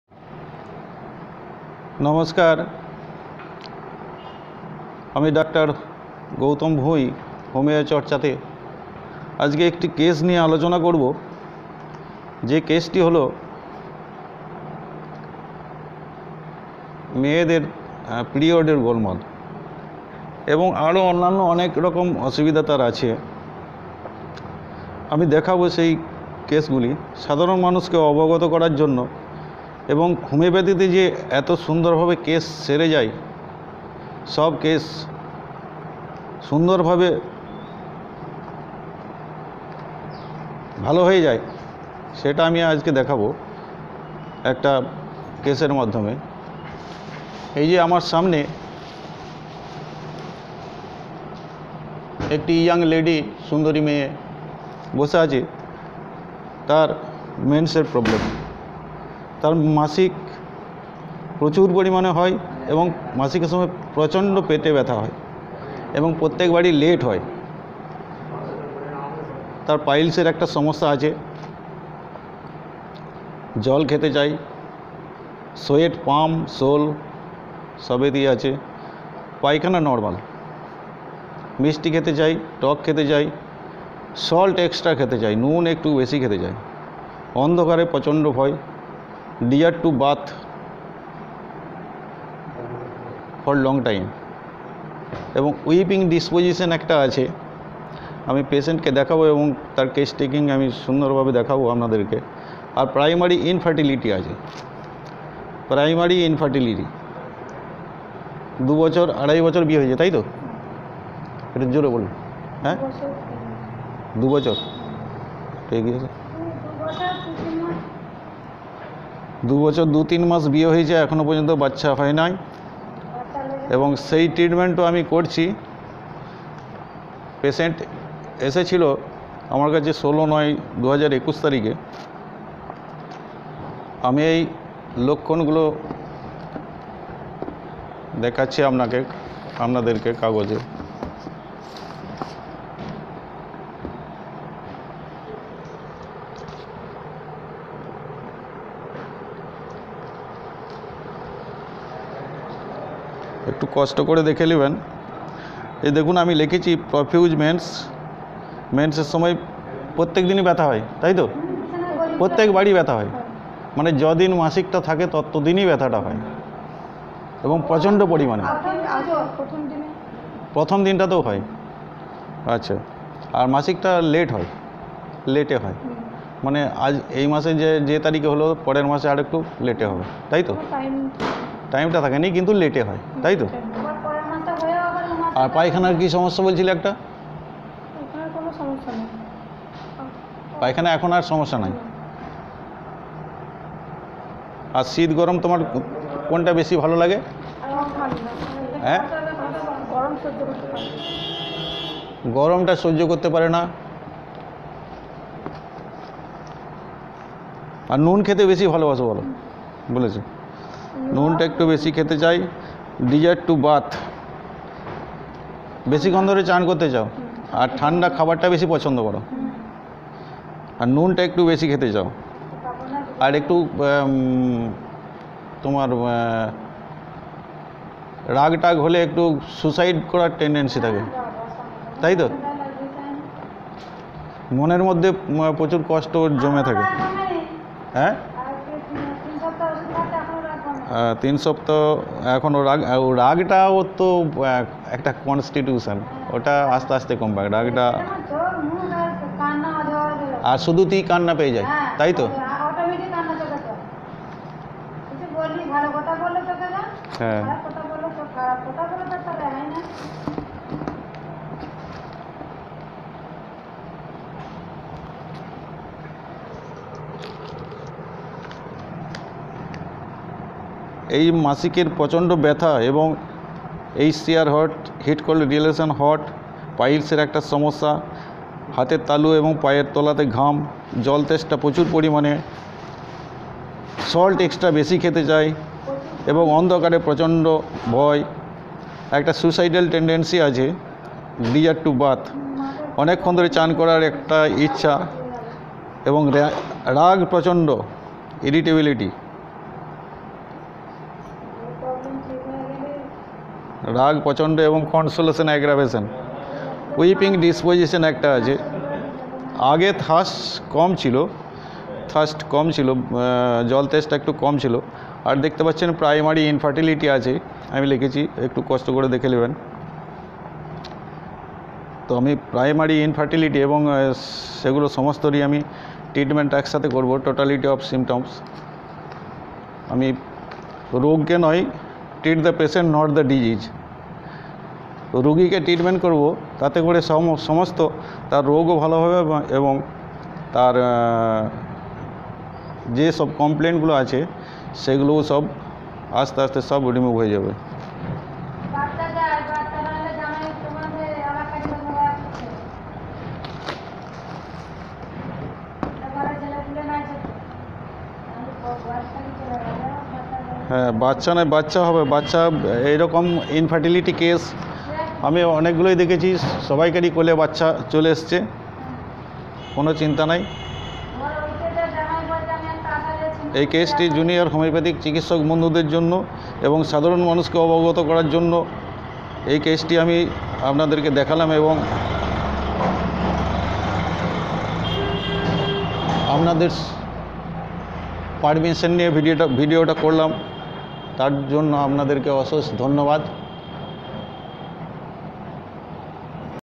Primary infertility & Painful mens-puls | বন্ধ্যাত্ব ও যন্ত্রণাদায়ক ঋতুস্রাব | Treatment Homeopathy
Primary infertility & Painful mens-puls | Treatment Homeopathy by Dr. Goutam Bhuin

Dr. Goutam Bhuin
Address - Jamuna Homeo Clinic
                 Bajitpur, Kenetty, Bankura